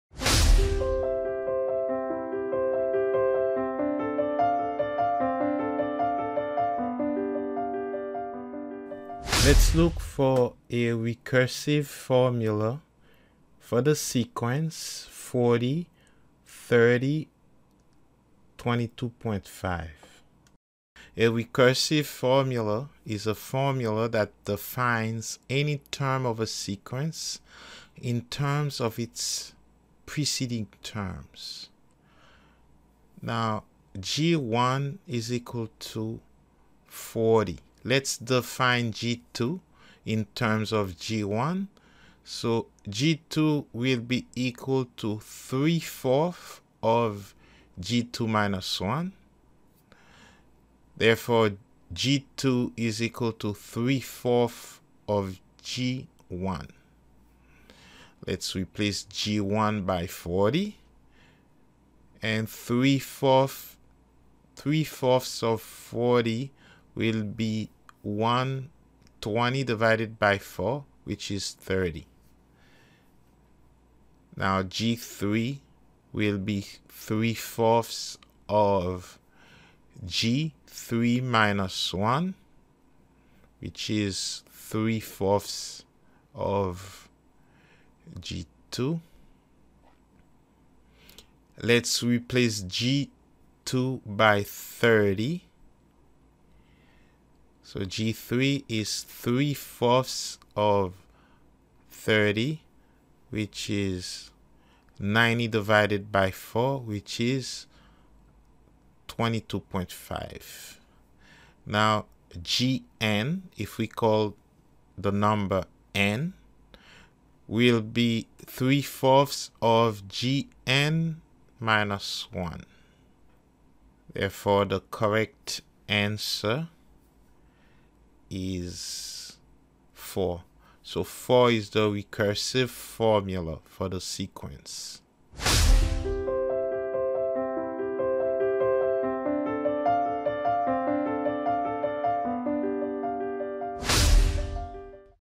Algebra 2 Review A recursive formula for the sequence 40,30, 22.5
Algebra 2 Regents Review, Algebra 2 Common Core Review